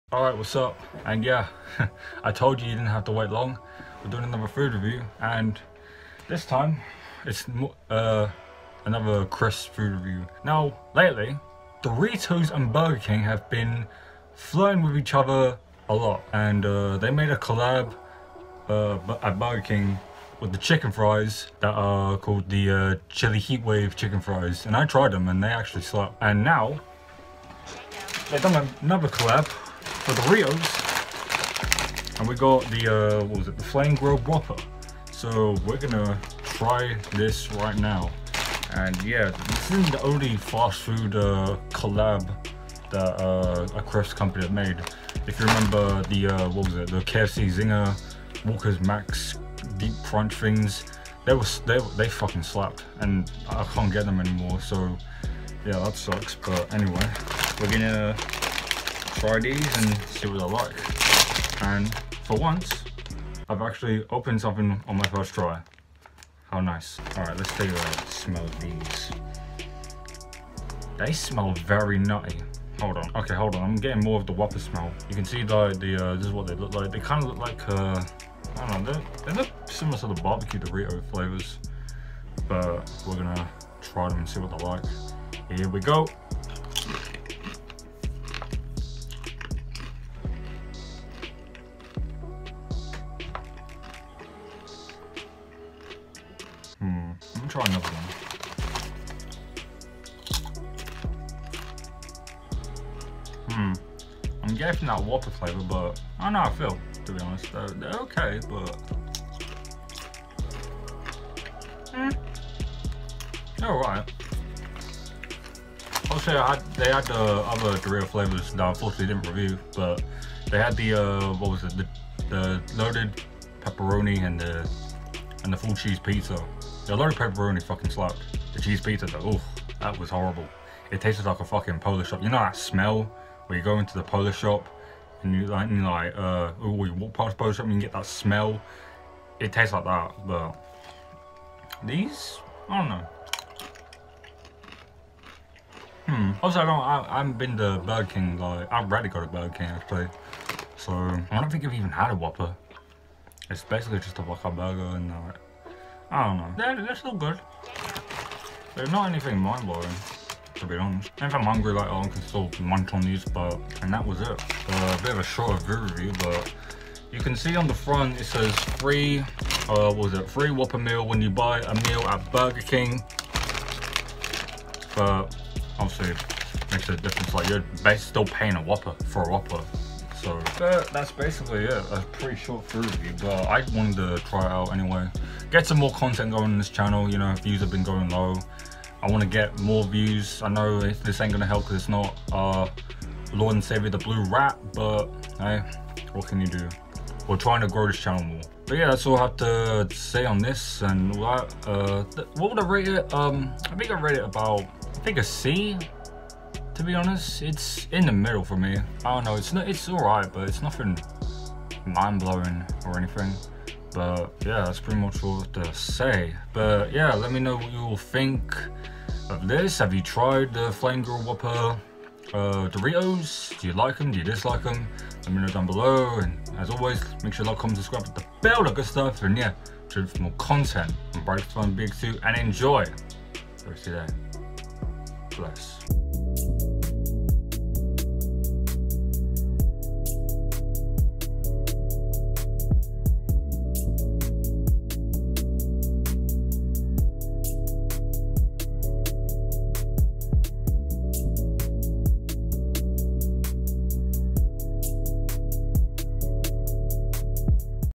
Can They Keep This Collab Up? | Doritos + Burger King Fame Grilled Whopper Flavour | Food Review
ANOTHER FOOD REVIEW?! Yessir! In this video, I will be reviewing the Doritos flame grilled whopper flavour as they have been collaborating with Burger King. Enjoy!

(Prod. jang0)